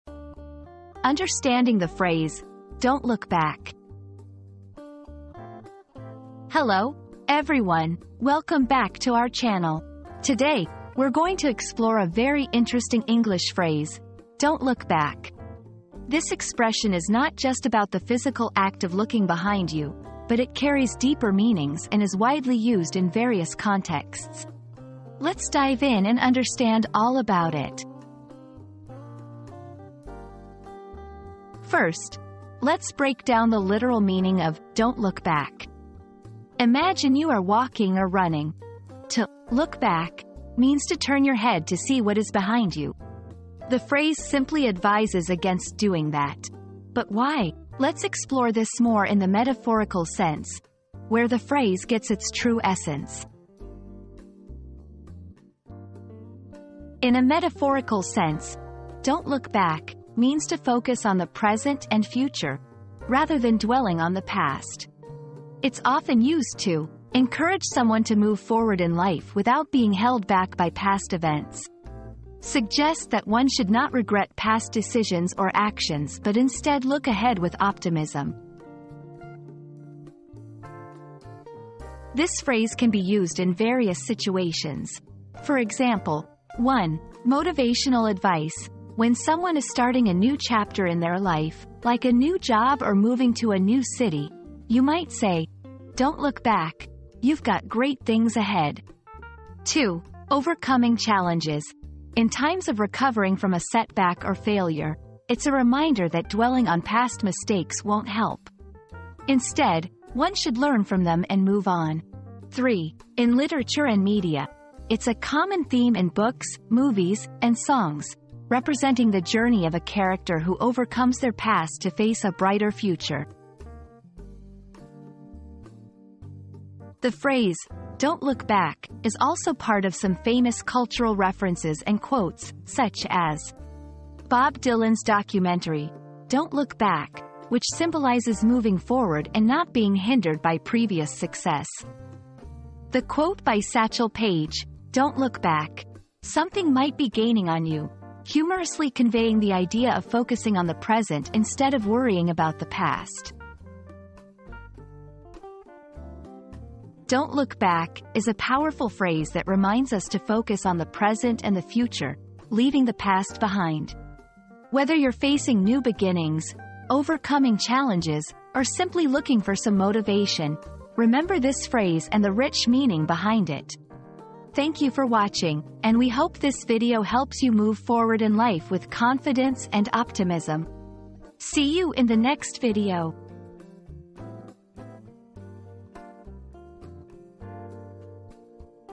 Understanding the Phrase "Don't Look Back"
Unlocking the Power of 'Don't Look Back' • Discover the profound meaning behind the phrase 'Don't Look Back' and learn how embracing this mindset can transform your life for the better.

00:00 • Introduction - Understanding the Phrase "Don't Look Back"
00:31 • Literal Meaning
00:56 • Metaphorical Interpretation
01:21 • Usage in Everyday Language
02:06 • Cultural References and Famous Quotes